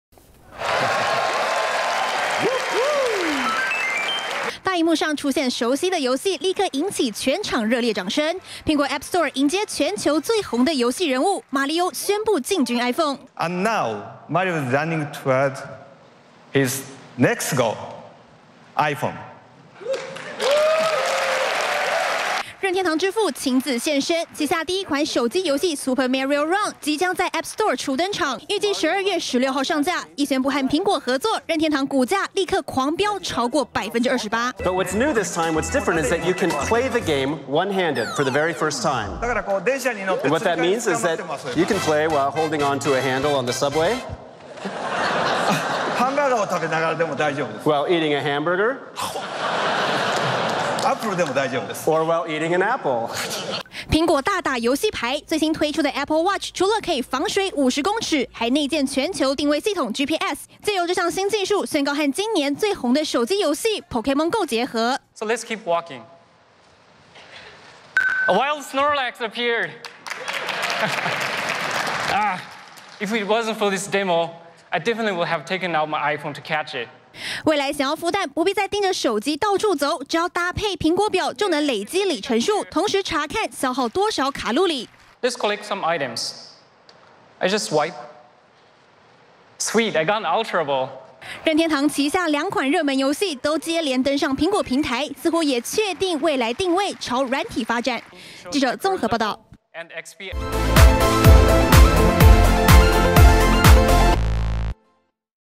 20160908中天新聞 蘋果大打遊戲牌 寶可夢、瑪莉歐登場
蘋果新品發表這次大打遊戲牌，新推出的apple watch，還結合今年最紅手機遊戲寶可夢，孵蛋、查看驛站都不是問題。另外蘋果還宣布與任天堂合作，讓經典遊戲超級瑪莉歐，首度登上手機遊戲平台，旗下兩款遊戲都跟蘋果產品結合，強強聯手，也讓任天堂股價立刻飆漲。